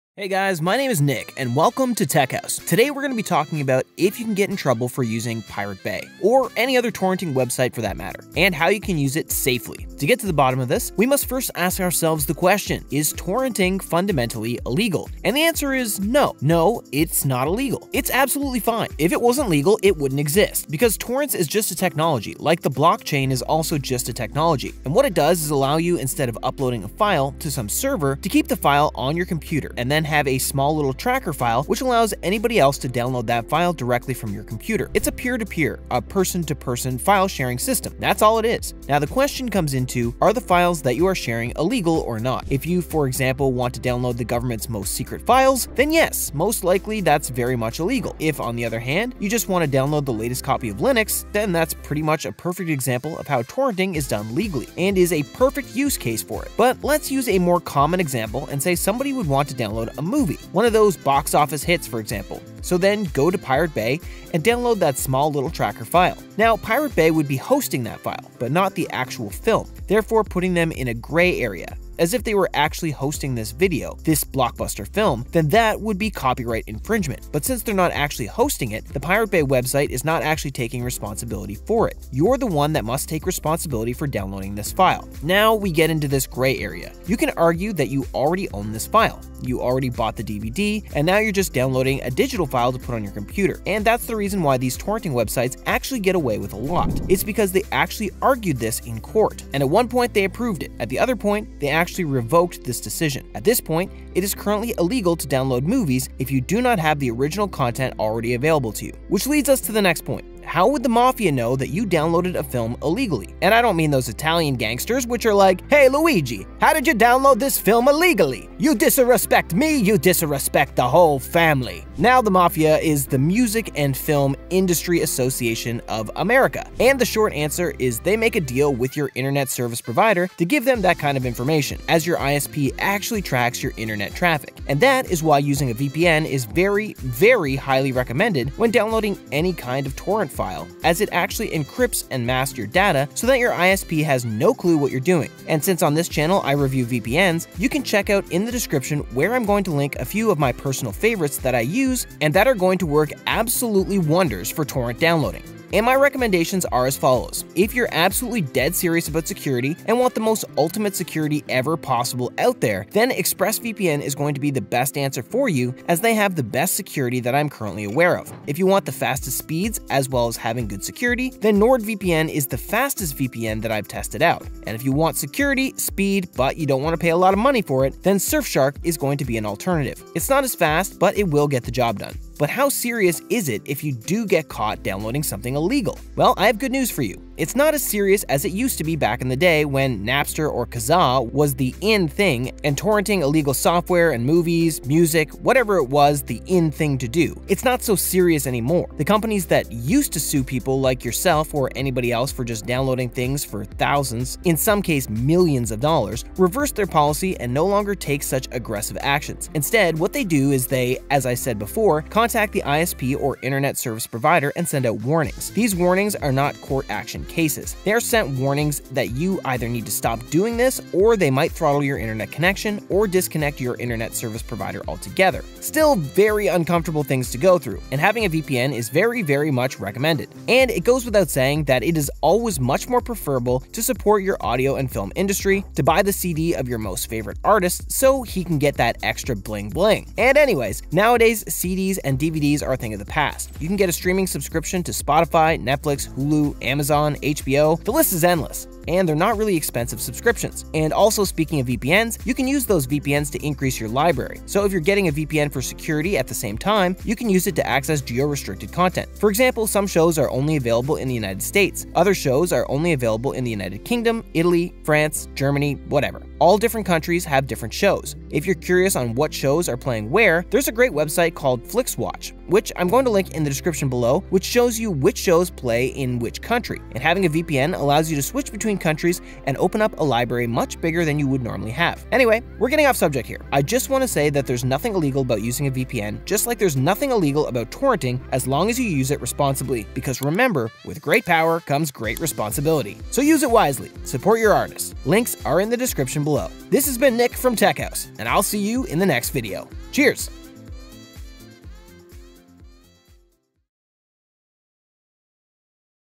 USE THE PIRATE BAY SAFELY 🏴‍☠️ How to Use The Pirate Bay and Enjoy Torrenting Anonymously?
🌐How to Use The Pirate Bay and Enjoy Torrenting Anonymously🌐

Torrenting, as a technology, is not inherently illegal; it's a peer-to-peer file sharing system akin to blockchain. The legality hinges on the content being shared. While downloading legal files, such as open-source software, is permissible, downloading copyrighted material, like blockbuster movies, enters a legal gray area.

Platforms like Pirate Bay, hosting tracker files but not the actual content, navigate this ambiguity. Recent legal developments suggest that downloading copyrighted material without owning the original content is currently illegal.

Protecting Yourself with a VPN

To avoid potential legal repercussions, it's crucial to protect your online activities. The Music and Film Industry Association of America (Mafia) often collaborates with internet service providers (ISPs) to track users' torrenting. Using a Virtual Private Network (VPN) becomes essential in this context.

A VPN encrypts and masks your data, preventing ISPs from monitoring your online activities. ExpressVPN, NordVPN, and Surfshark are recommended VPNs, each catering to different preferences, whether security-focused, high-speed, or cost-effective.

Legal Implications and Responsible Usage

While consequences for illegal downloading have become less severe compared to the past, caution is still advised. Companies no longer pursue aggressive legal action but issue warnings through ISPs. These warnings may lead to internet throttling or disconnection. Utilizing a VPN remains a proactive measure to safeguard against such warnings.

Emphasizing legal alternatives, the narrative encourages supporting the entertainment industry through subscriptions to streaming services like Spotify, Netflix, and others. Additionally, the use of VPNs can enhance your content library by accessing geo-restricted content legally. In conclusion, the key message is that neither VPNs nor torrenting are inherently illegal, provided they are used responsibly and legally.

Hope you enjoyed my USE THE PIRATE BAY SAFELY 🏴‍☠️ How to Use The Pirate Bay and Enjoy Torrenting Anonymously? Video.